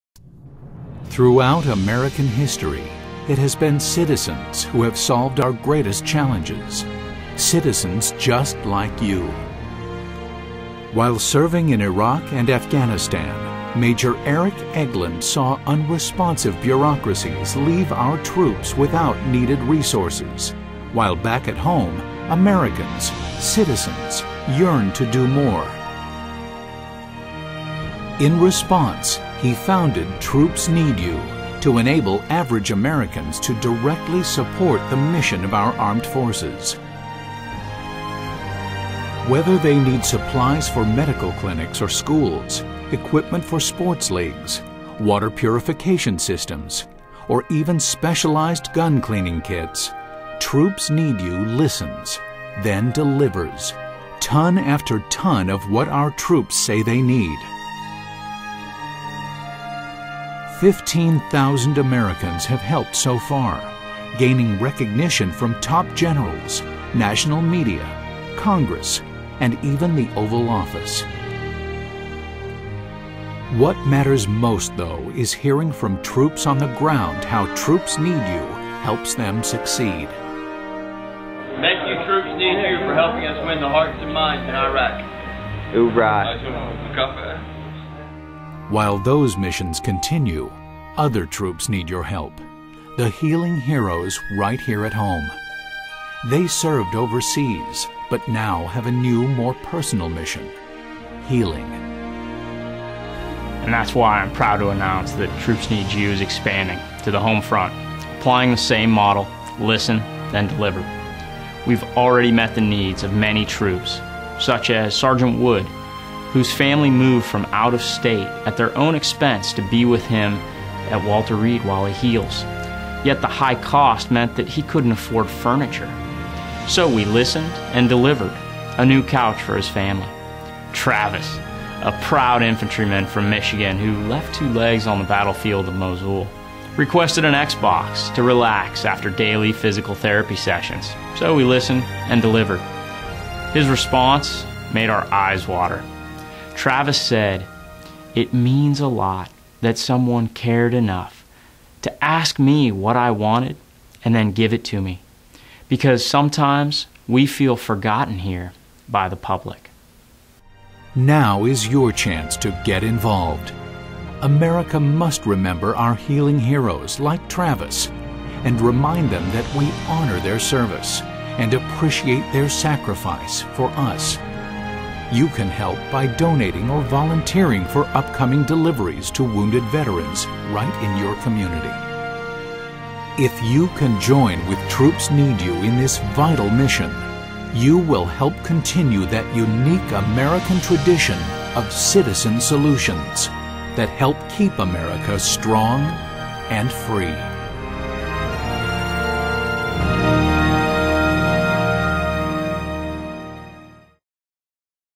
Troops Need You Expands to the Homefront
Want to go beyond the care package and bumper sticker to help our healing warriors?  Troops Need You is helping wounded soldiers and Marines after their service and sacrifice.